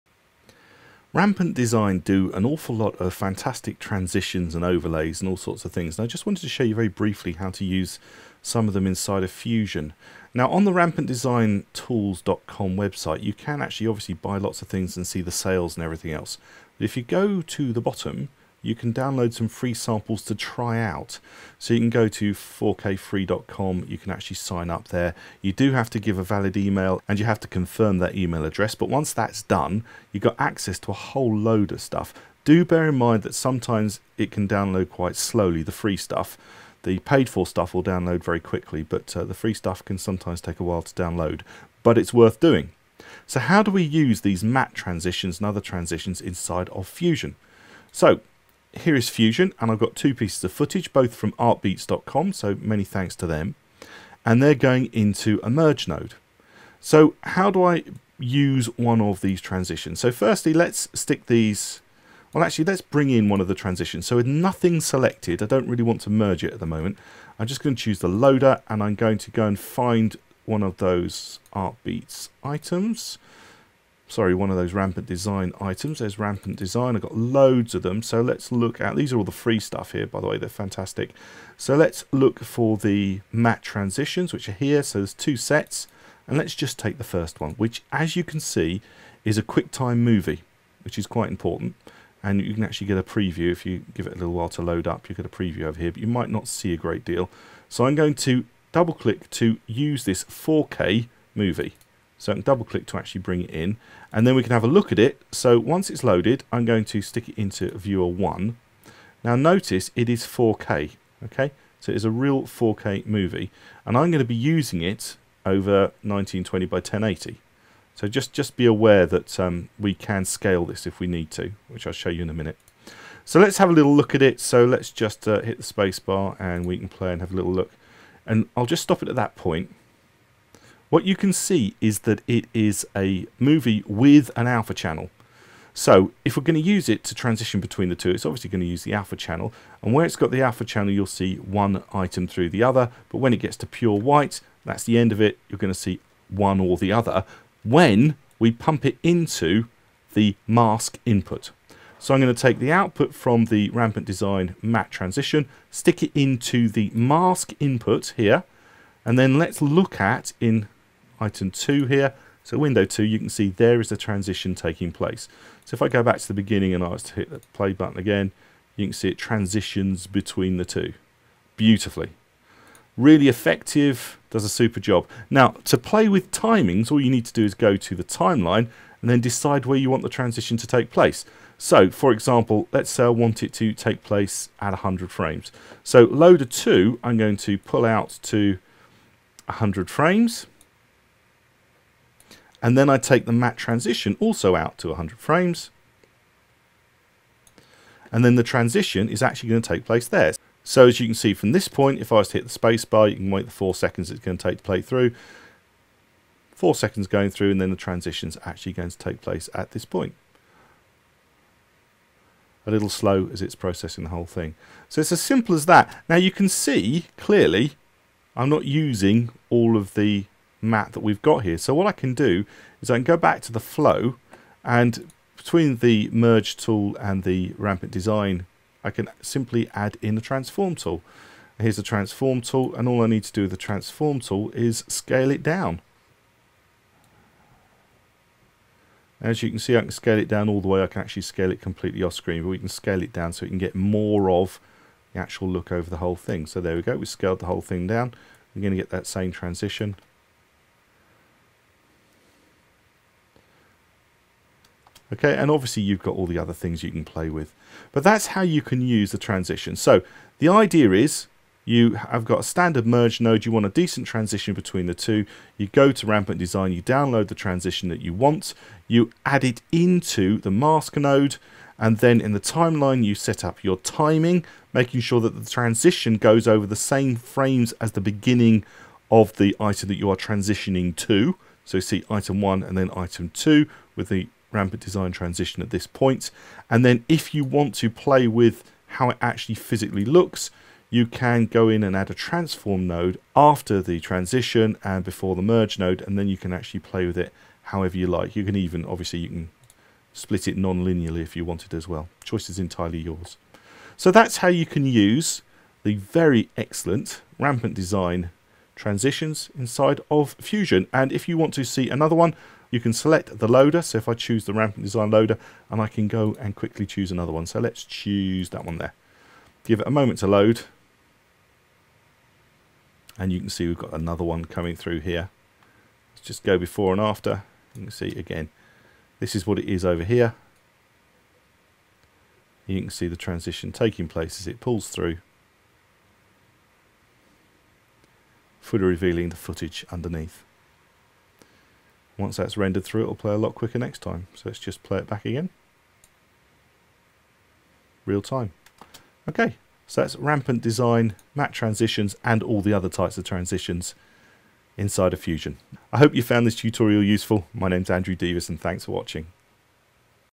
How to use Rampant Design Matte Transitions in BlackMagic Design Fusion
Andrew Devis from @tale2tell_media shows us how to use @RampantDesign Matte Transitions inside of @blackmagic_news Fusion.

►Check out More Free Training (Fusion, After Effects, Premiere, Resolve, Vegas) from Andrew Devis